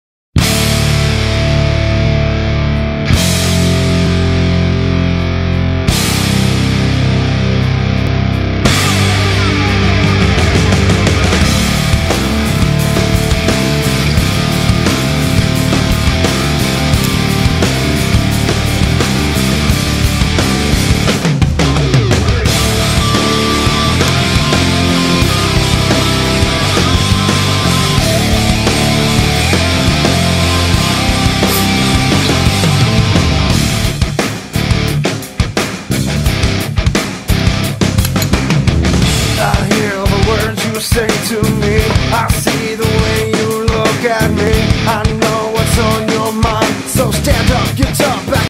Sanrenmu T08 - hard tactical use
How to use the Sanrenmu T08 in a tactical fashion. Opening a great beer: the Leffe Triple!